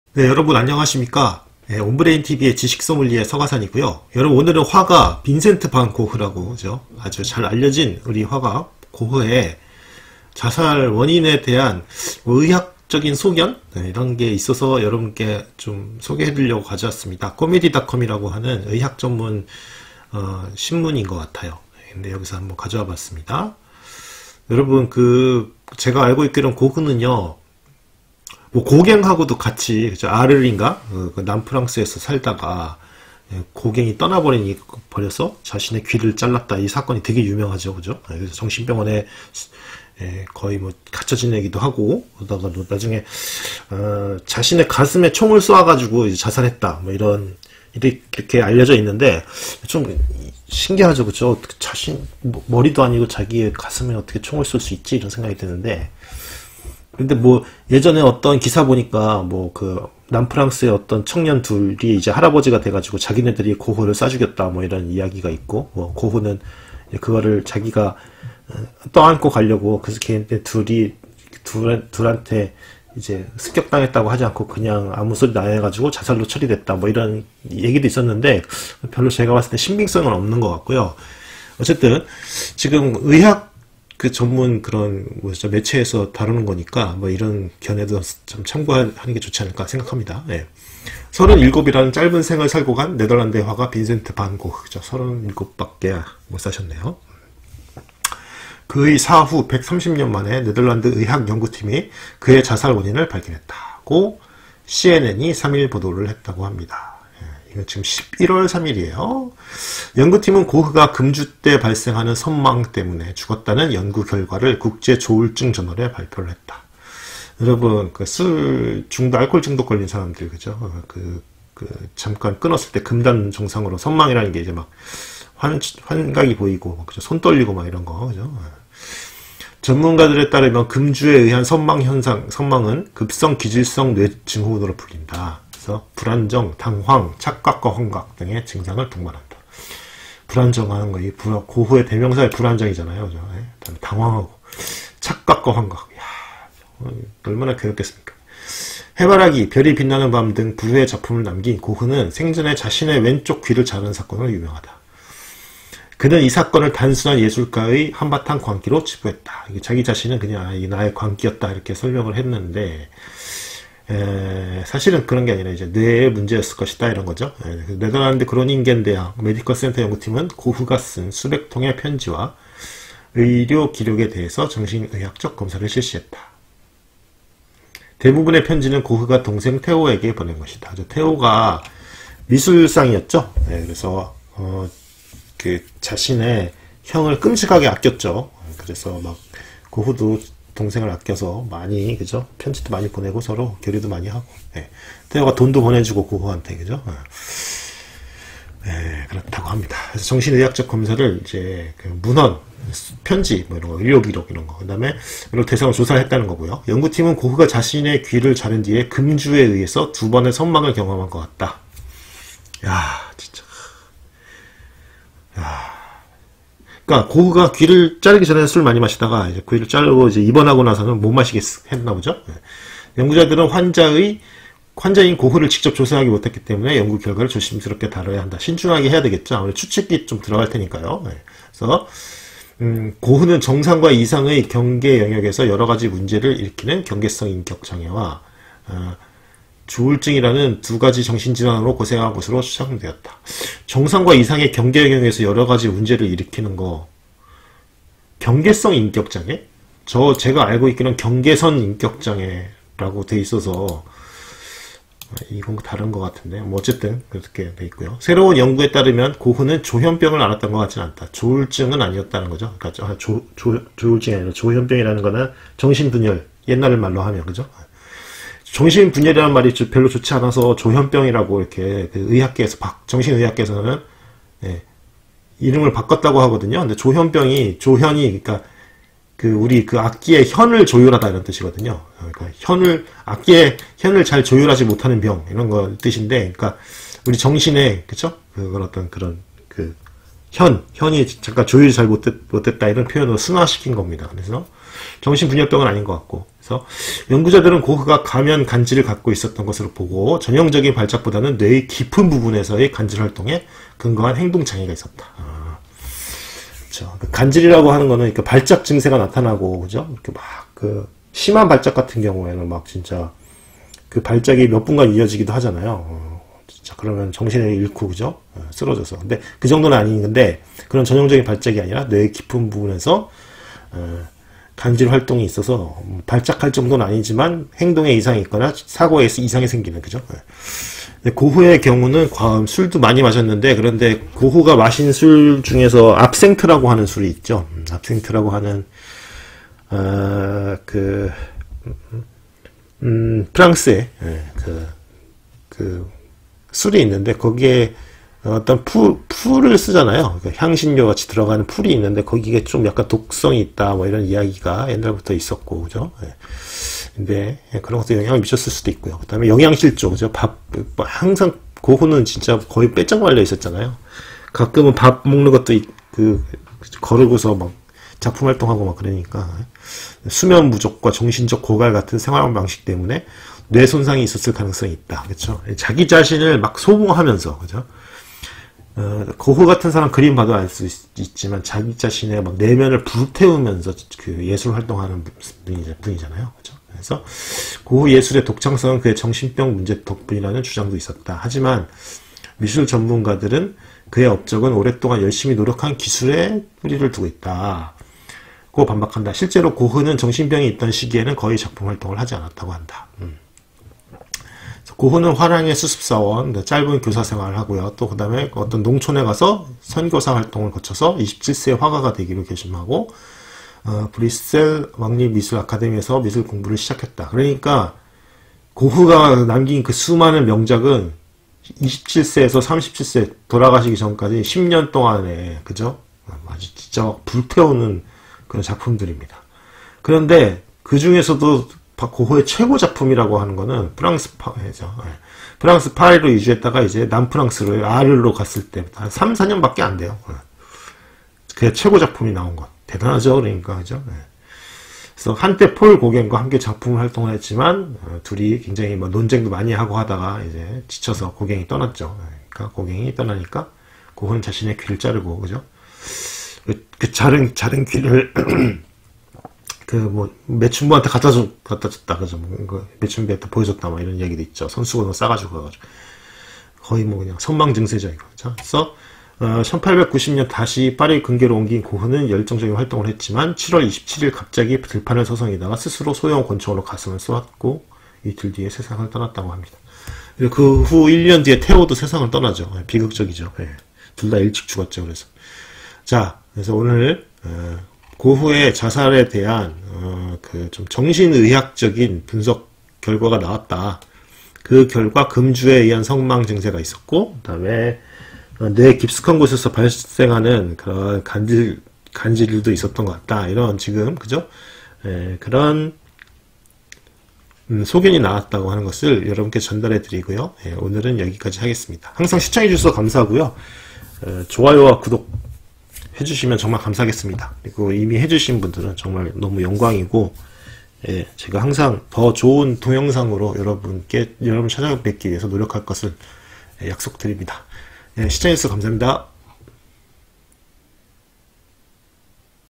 고흐 자살에 대한 정신의학적 견해
네덜란드 정신과 의사들의 공식 견해라고 합니다!